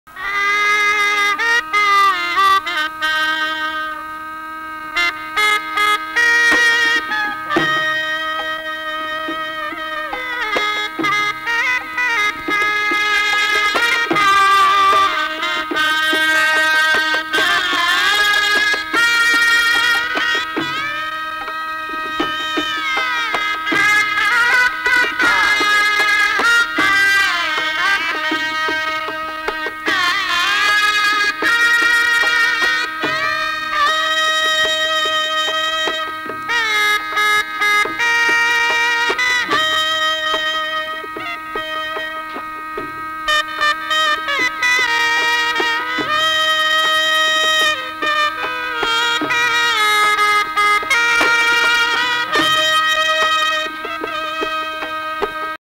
Ustad Bismillah Khan | Shehnai Maestro | शहनाई वादन...
Prasar Bharati Archives remembering Ustad Bismillah Khan, musician who played the shehnai, a ceremonial oboe like North Indian horn, with such expressive virtuosity that he became a leading Indian classical music artist.